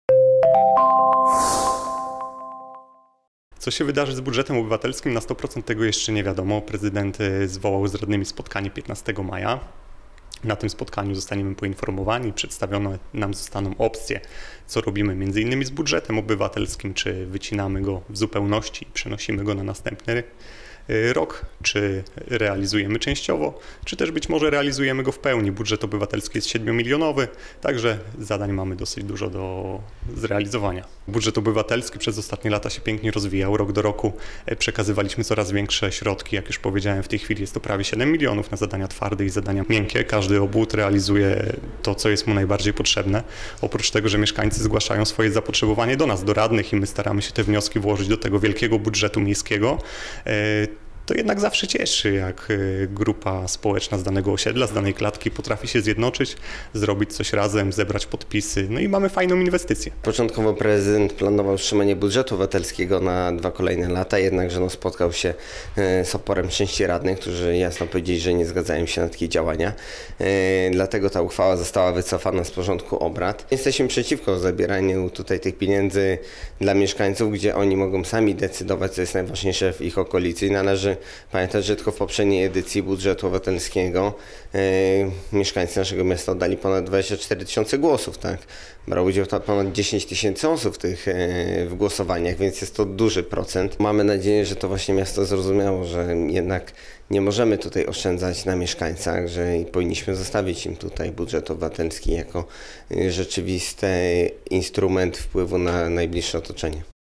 Mieszkańcy nie odpuszczają Budżetu Obywatelskiego
„Zamrożenie” Budżetu Obywatelskiego zostało… „zamrożone” Mieszkańcy nie odpuszczają i walczą o finansowanie swoich pomysłów na zamianę ulic, parków i najbliższej okolicy. Zgodnie z zapowiedzią prezydenta Jacka Wójcickiego z ostatniej sesji rady miasta, w sprawie decyzji dotyczących Budżetu Obywatelskiego, planowane są rozmowy z radnymi, a będą one dotyczyć zmian uchwały o trudnej sytuacji finansowej miasta.